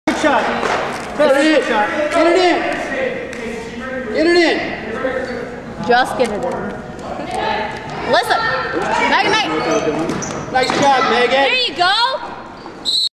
Just Get It In - Just about perfect
Johnsburg Sophomore Girls basketball runs a near perfect "Just Get It In"